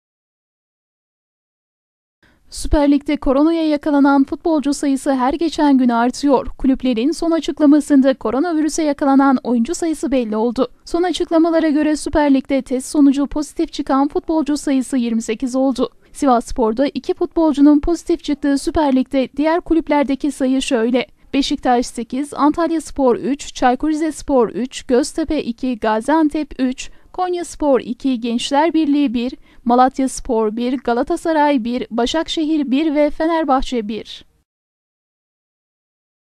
KOVİD-19 SÜPER LİGİ TEHDİT EDİYOR!
Süper ligde koronaya yakalanan futbolcu sayısı her geçen gün artıyor.